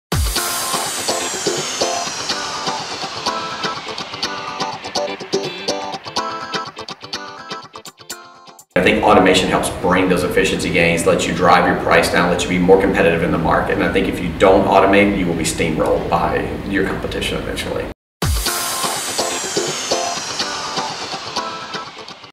What are the Benefits of Automation?
Ray Kimble, General Manager OF Sales & Marketing at ABIS, Inc details the Benefits of Automation for Businesses.